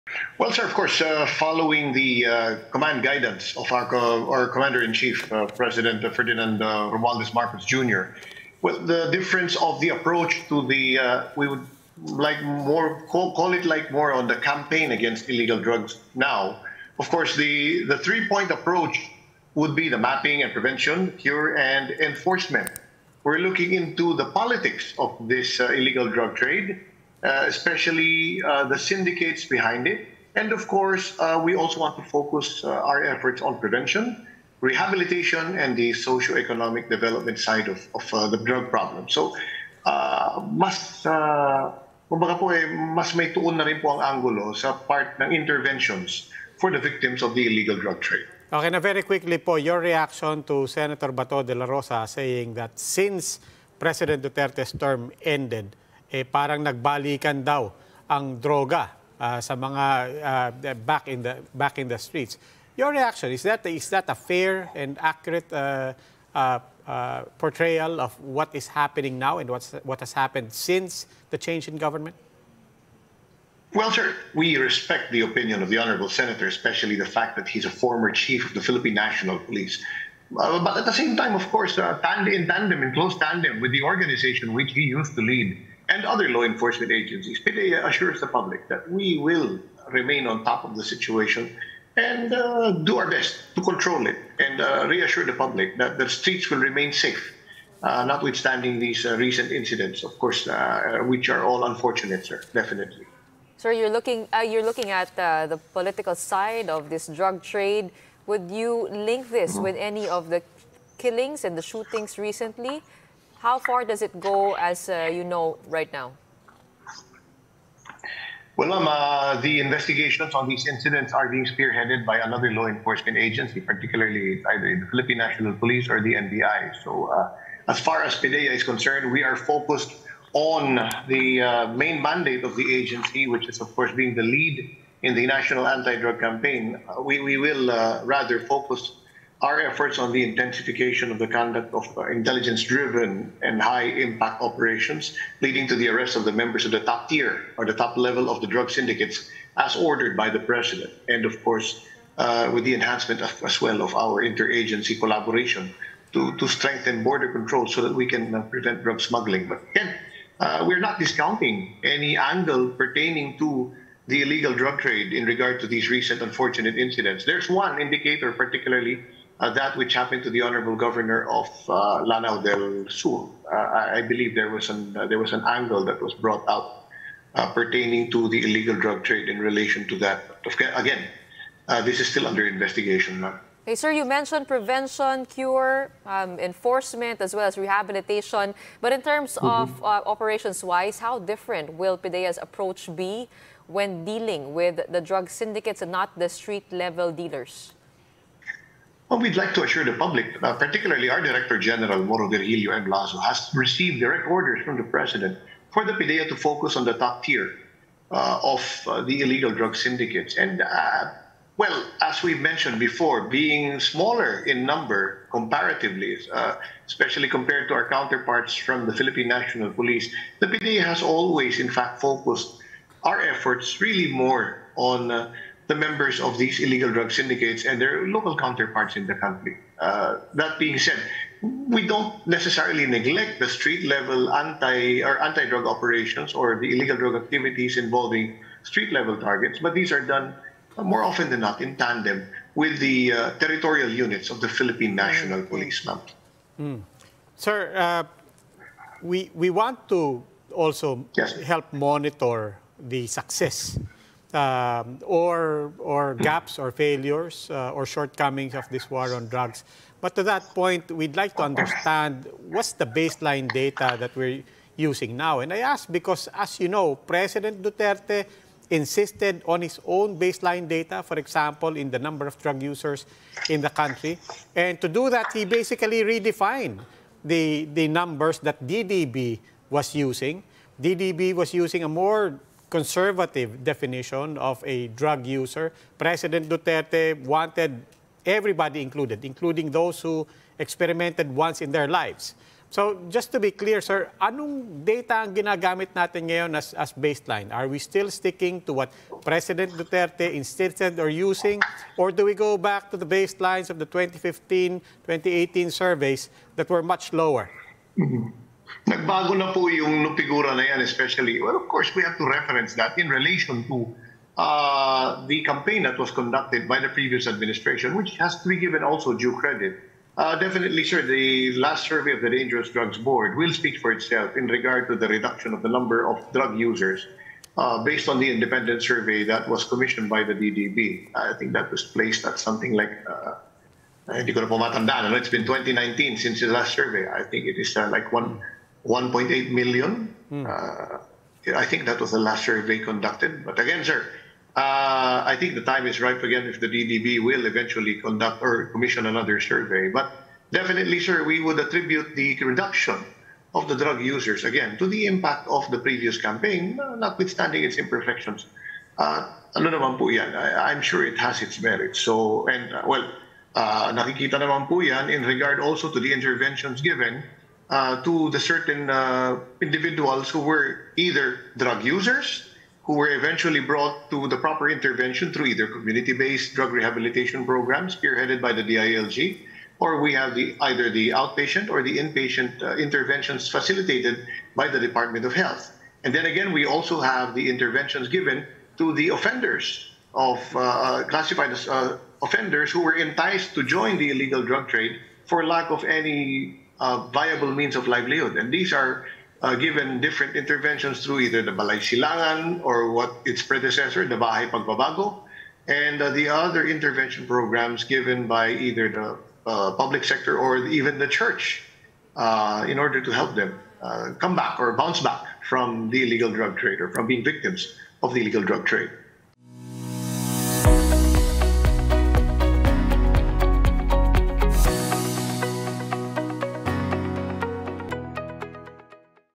PDEA ordered to capture larger drug cartels
President Ferdinand "Bongbong" Marcos Jr. has ordered the Philippine Drug Enforcement Agency (PDEA) to go after top-tier drug syndicates in the country.

PDEA says it focuses its efforts against members of drug syndicates and their local counterparts.